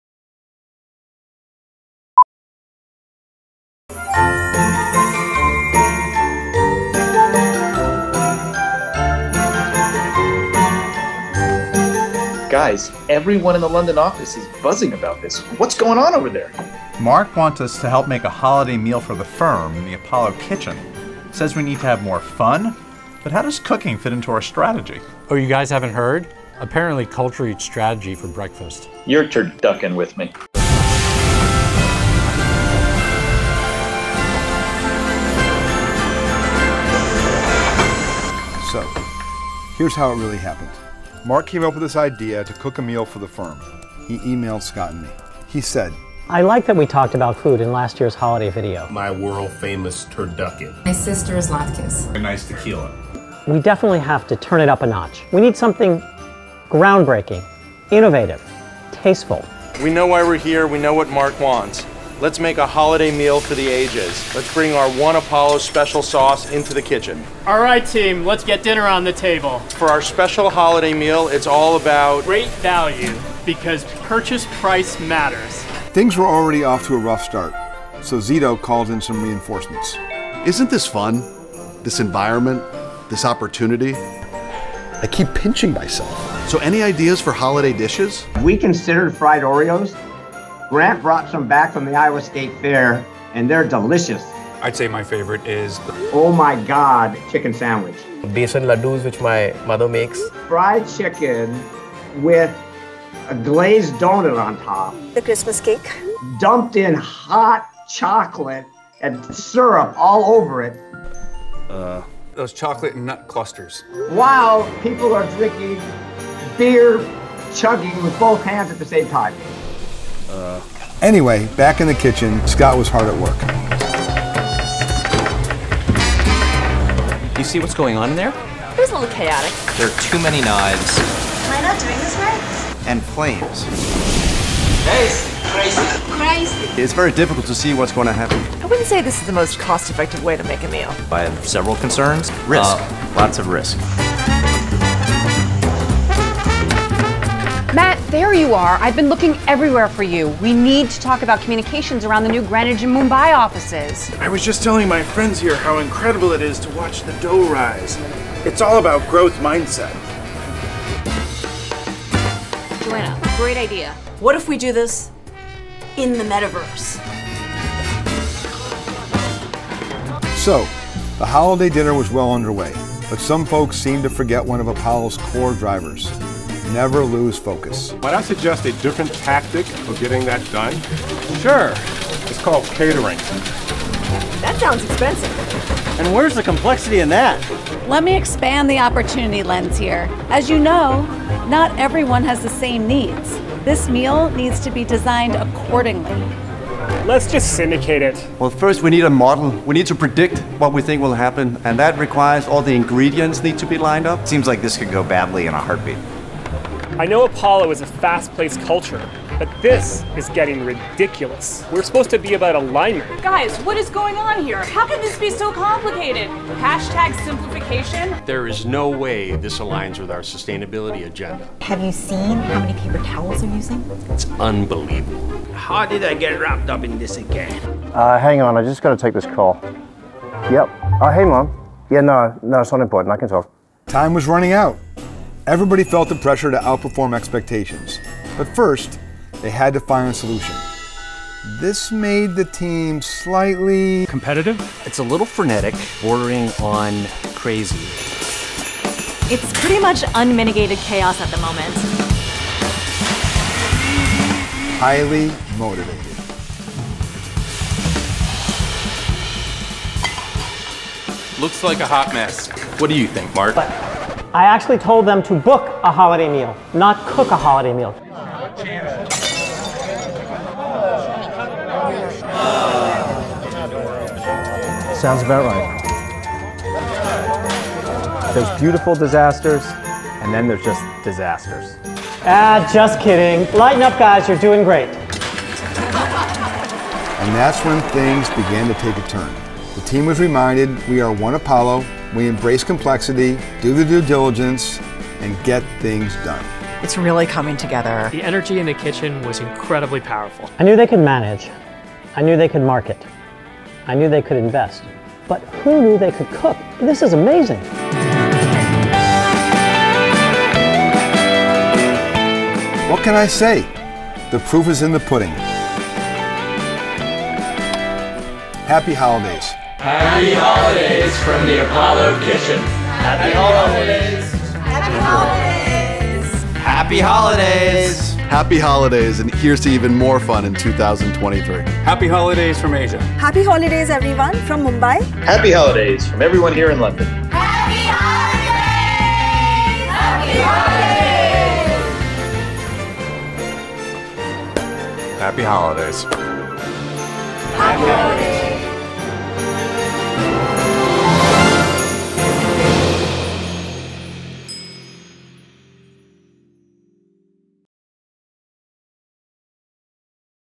Season's Eatings from Apollo!
Season’s Eatings! We’re closing out the year in our beloved Apollo kitchen. Wishing everyone happy holidays from all of us.